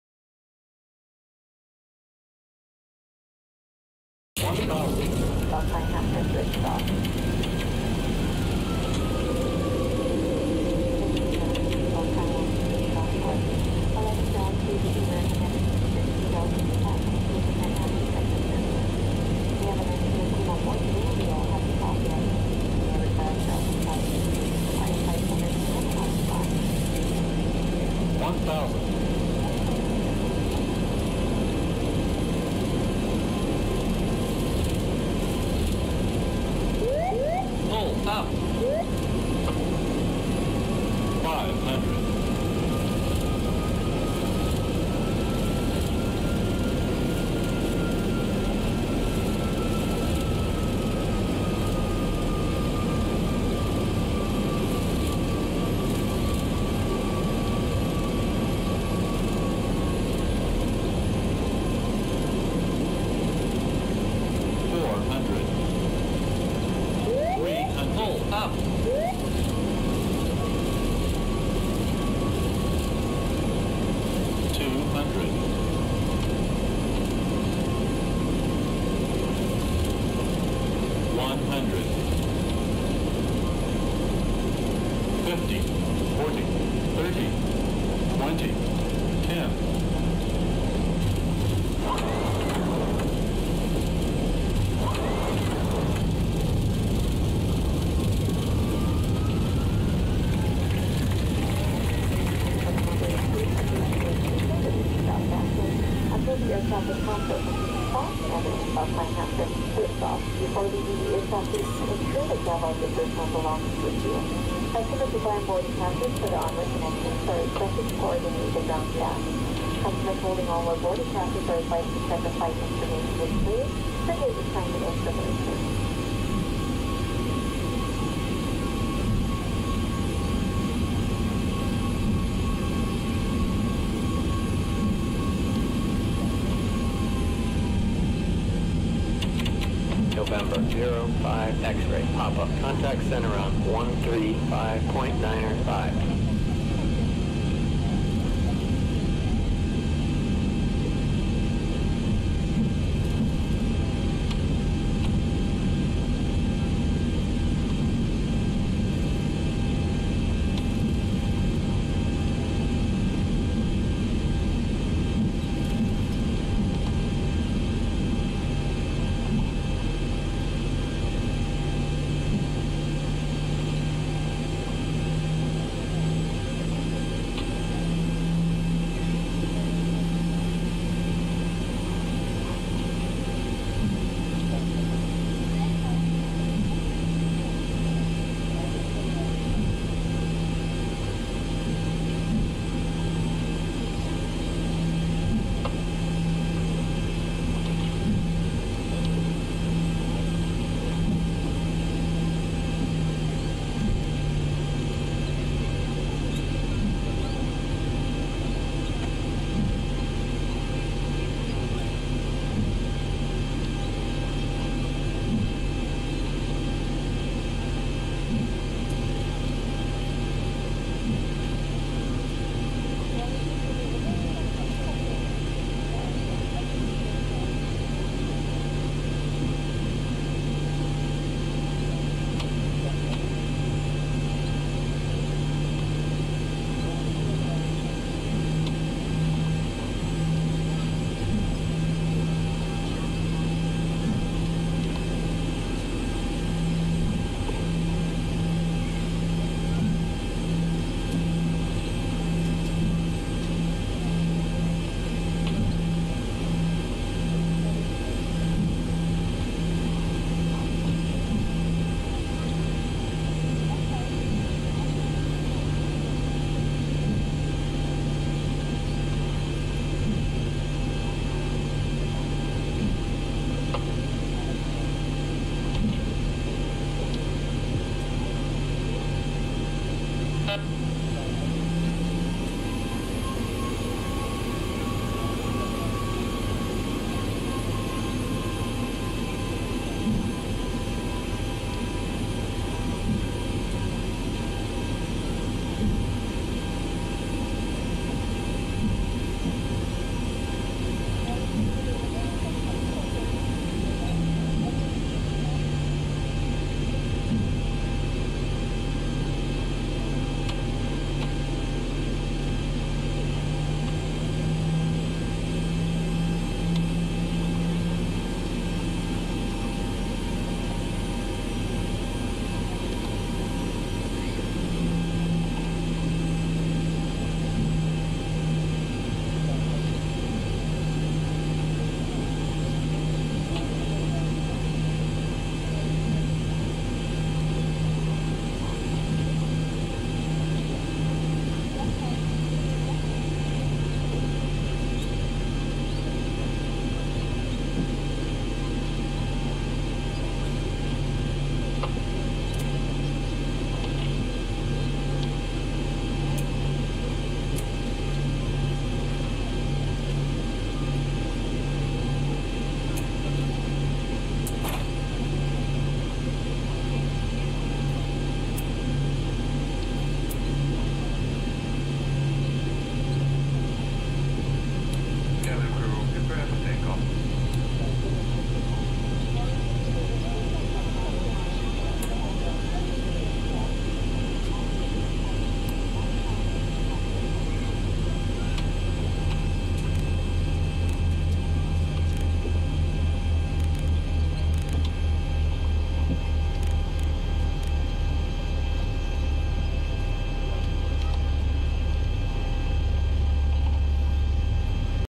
How Not to Go Around but Not Crash in Severe Typhoon HATO
HONG KONG ARRIVAL INFORMATION
ARRIVAL RUNWAY 07L
RWY SFC WET
SIG WS FCST AND MOD TURB FCST
WIND 150 DEG 26 KT
VISIBILITY 10KM
CLOUD FEW 1500FT SCT 2200FT
IN PASSING SHOWERS
TEMPERATURE 28 DEWPOINT 25
QNH 0996 HPA
EXP SIG TAILWIND ON BASELEG
TREND TEMPO VIS 2500M HVY SHOWER